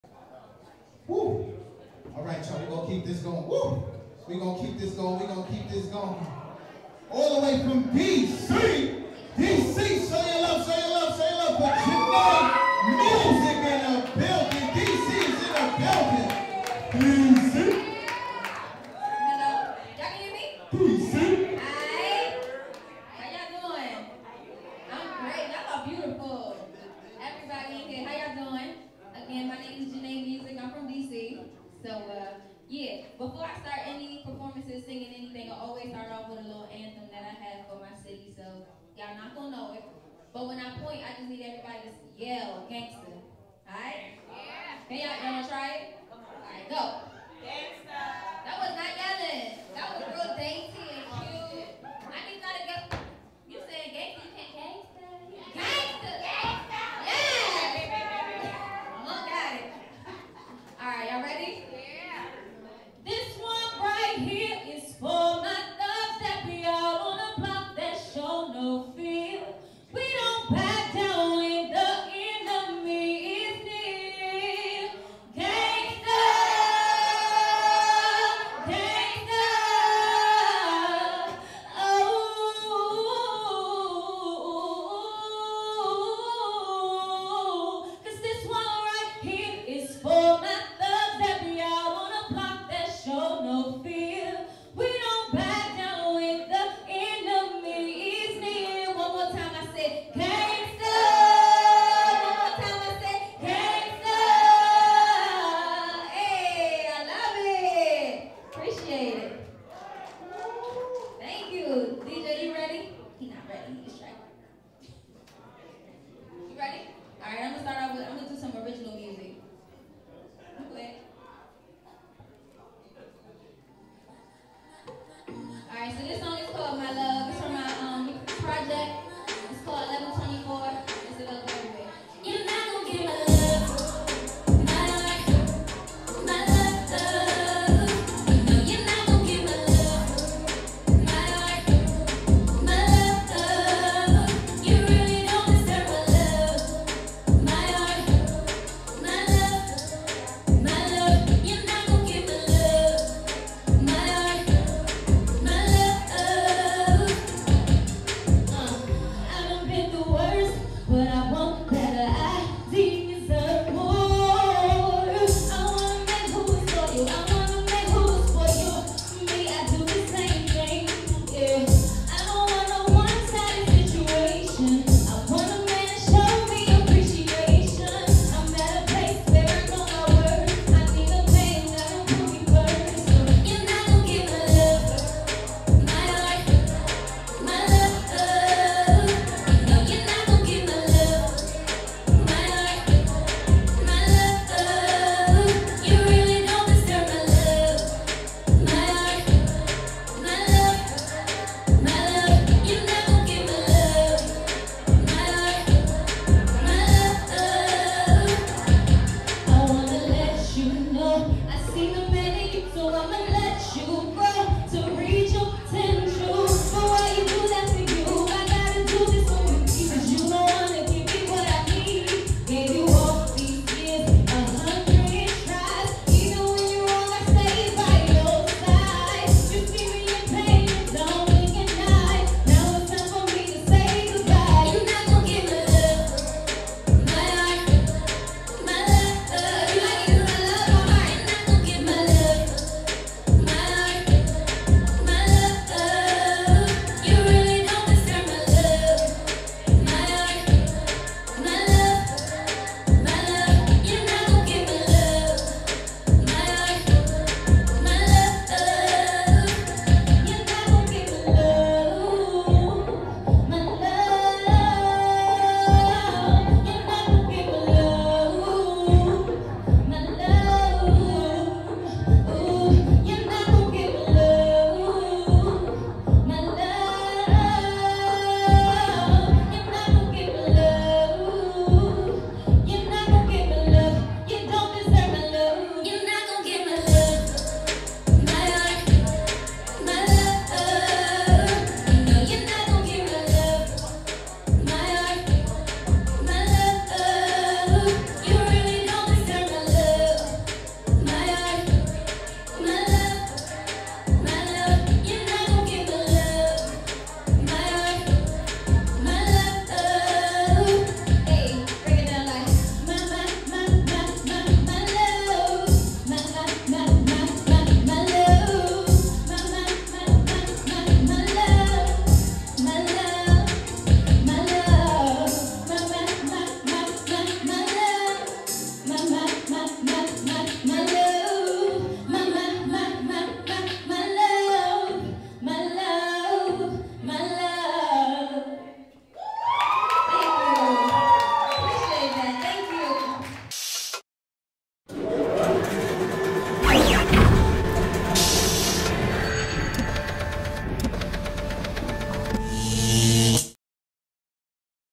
Blaze the Stage Mania 20/JANAE MUSIC
ALL STAR CREATIONS Presets: Blaze the Stage Mania 20 with JANAE MUSIC (Perfect Verse Over A Tight Beat)@JanaeMusic_ from Washington, DC
This is a platform where upcoming artists and models be discovered, and hopefully become their own legend.

About this Event:
Blaze the Stage Runway Fashion & Concert @
Manley's Eat the Bone
1764 Pitkin Ave.

Complimentary Jamaican food & finger food